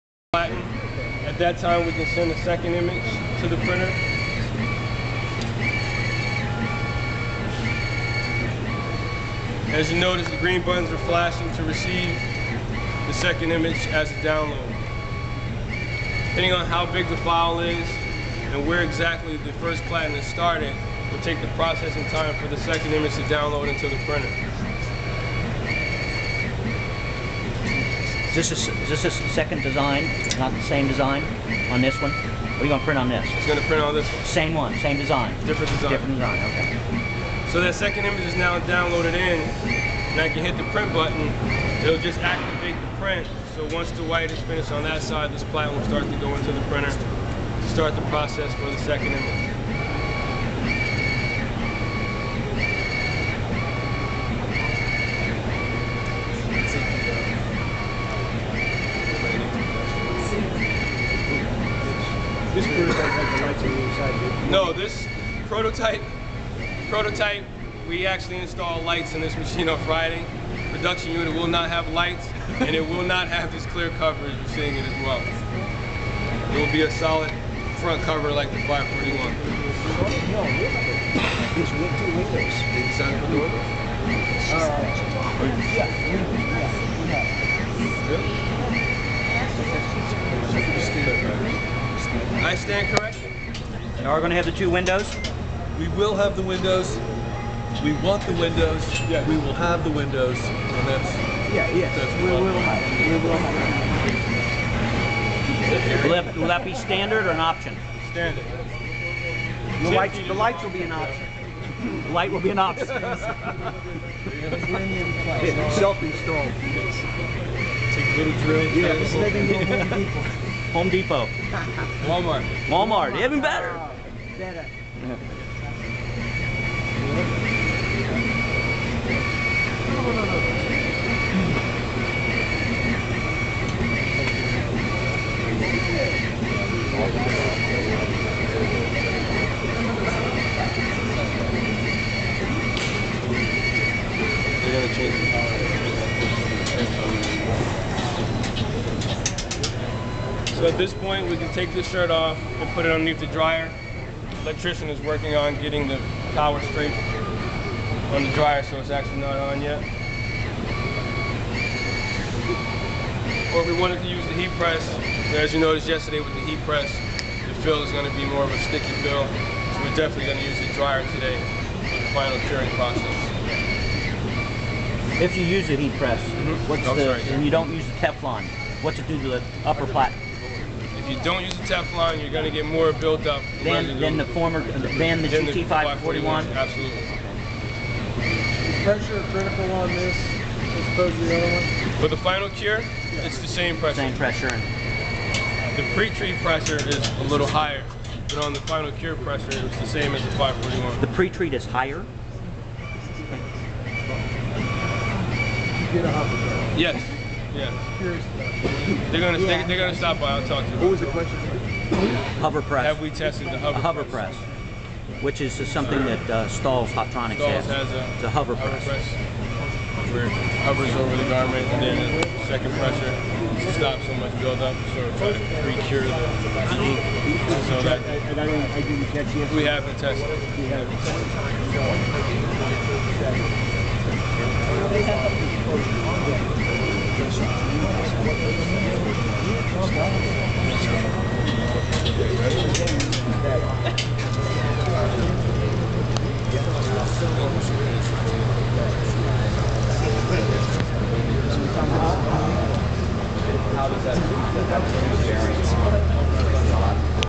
Brother GT-782 Direct to Garment White Ink Printer Preview
This movie was taken at the SGIA by Jay Jessup in conjunction with Direct2Shirt with his camera showing the basic operation of the Brother GT-782 white ink direct to garment printer. You can get a basic idea of its operation and ease of use. In a real life production situation you would be producing around 30-60 shirts per hour depending on the print mode. You can learn more by visiting and going to the Brother Digital Garment Printer forum.